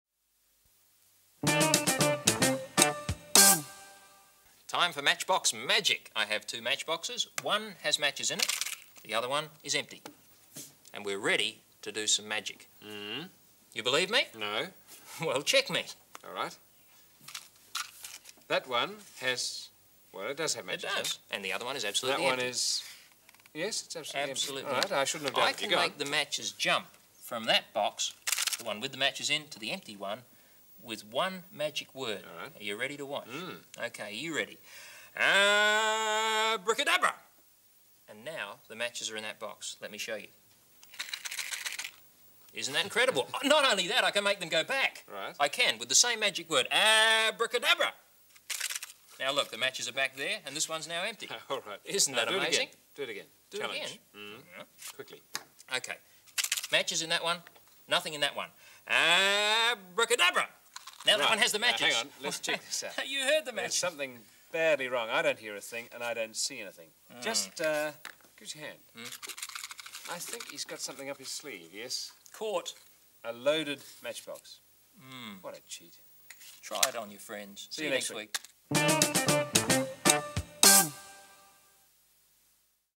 Magic, Invisible, Flying Matchsticks
Deane and Rob hunt for some magic, disappearing matchsticks. You will want to try this on your friends!